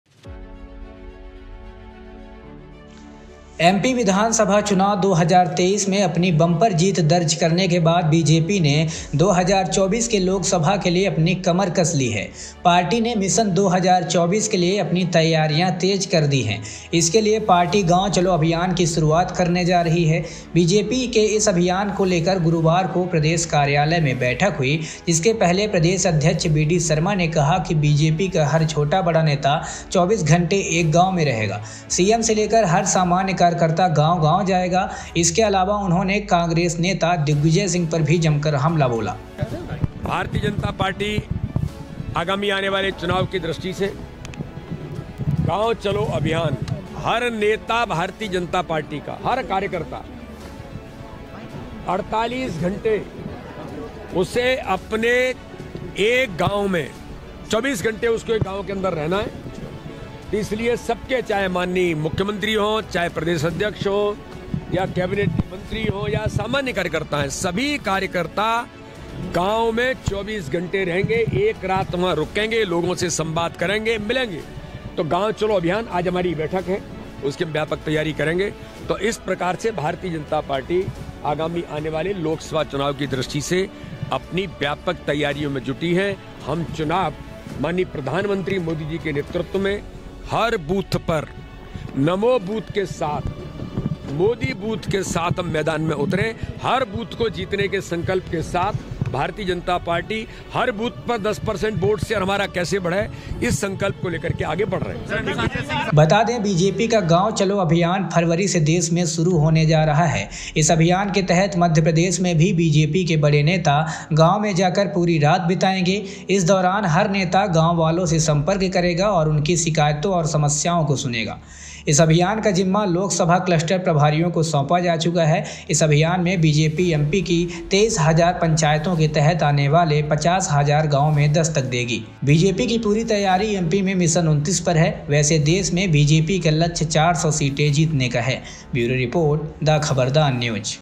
BJP के इस प्लान से Congress में मच गया हड़कंप ?
Before the Lok Sabha elections, BJP is attacking 'Gaon Chalo Abhiyan'. Under this campaign, Chief Minister Dr. Mohan Yadav, state organization leader Hitanand Sharma, state president Video Sharma and contact leaders of BJP will spend 24 hours in the village. BJP state president Video Sharma made this announcement on the occasion of Navgraha conference at the state BJP office.